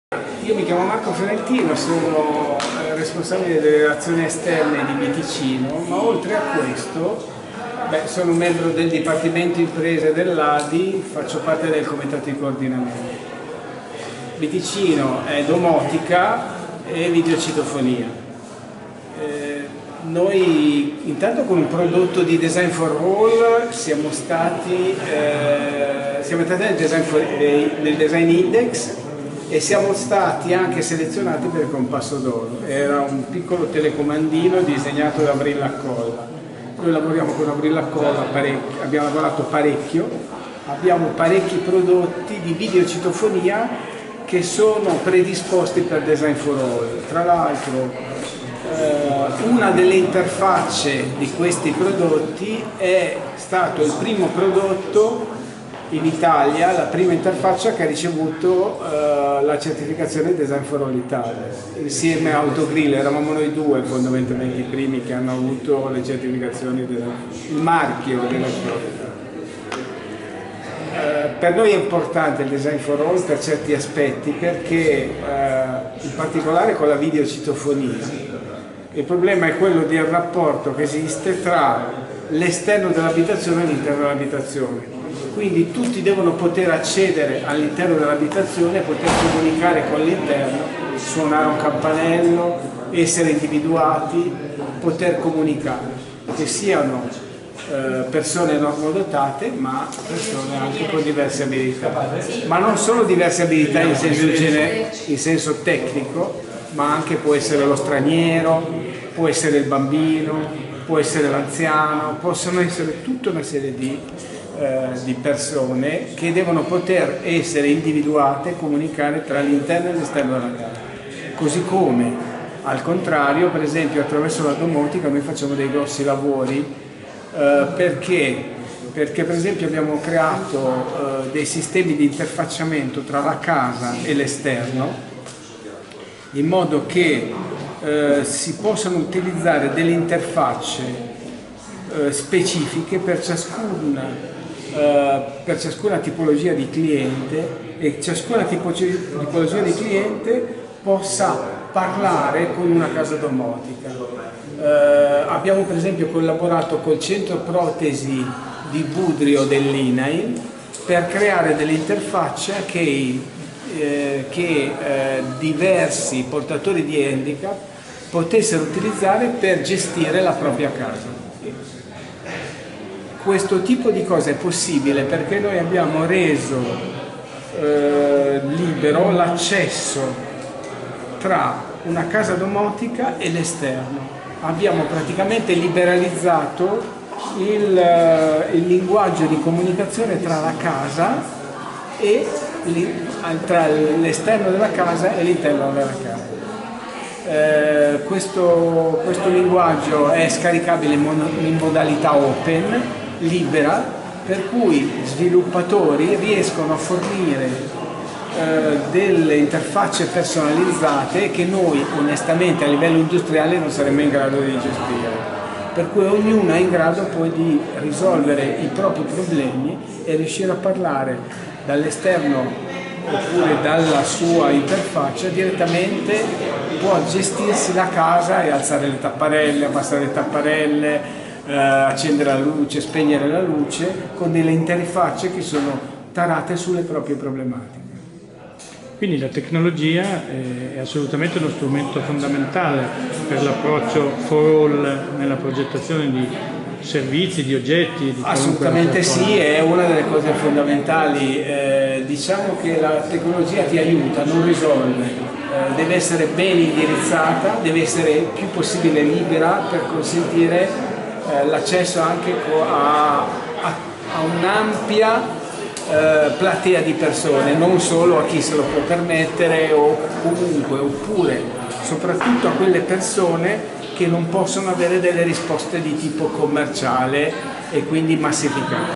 Bticino al 50esimo anniversario dell'Ordine degli architetti di Varese
lifeforall-blog intervista Marco Fiorentino di BTicino.